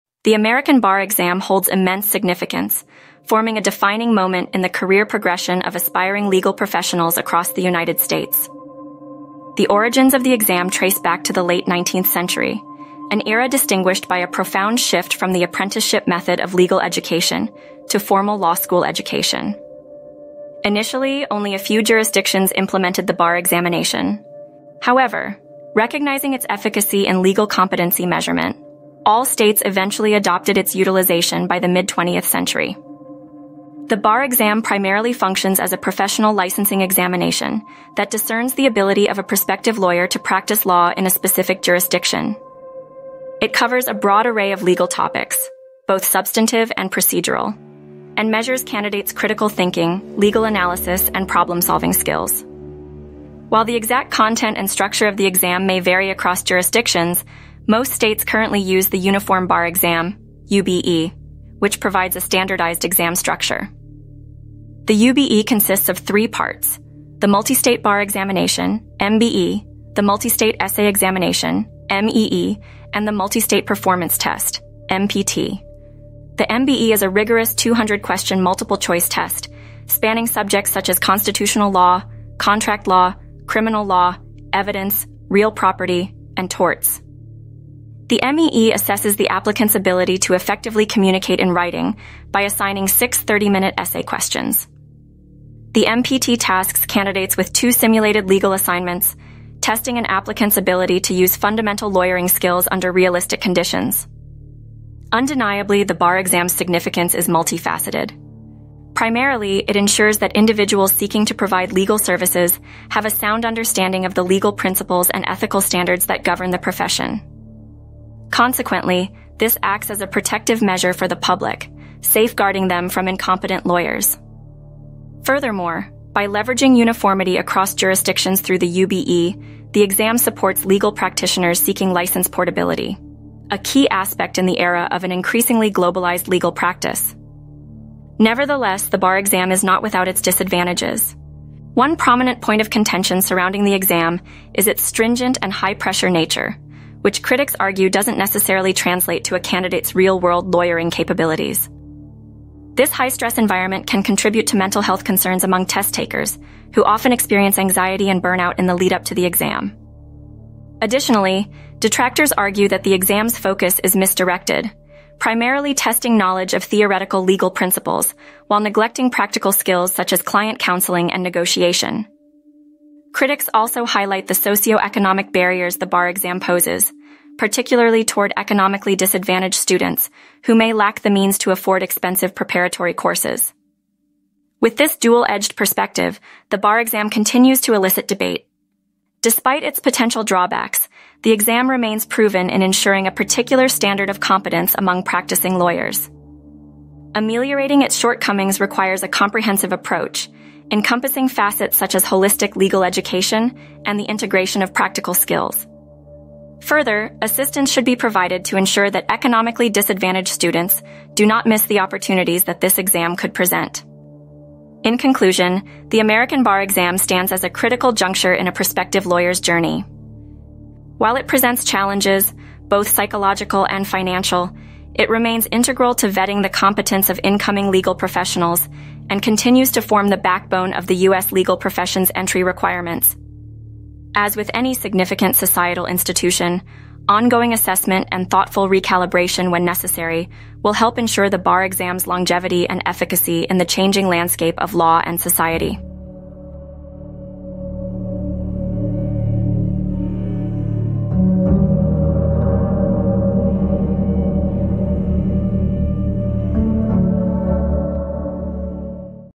The American Bar Exam
In this informative video, we delve deep into the world of the American Bar Exam. Get an inside look at the intimidating legal test that all law graduates must navigate to practice law in the United States. Understand its complex structure, study the multiple sections including legal principles, the laws of evidence, contracts, and constitutional law. Gain insights from industry experts and experienced lawyers as they share their tips and strategies on how to succeed. Learn more about the grading process, the varying requirements between states, and the rigorous preparation it entails. Whether you're a law student prepping for the big test or just interested in the fascinating world of law, this comprehensive guide to the American Bar Exam is a must-watch. Join us as we demystify this challenging hurdle every aspiring lawyer must overcome.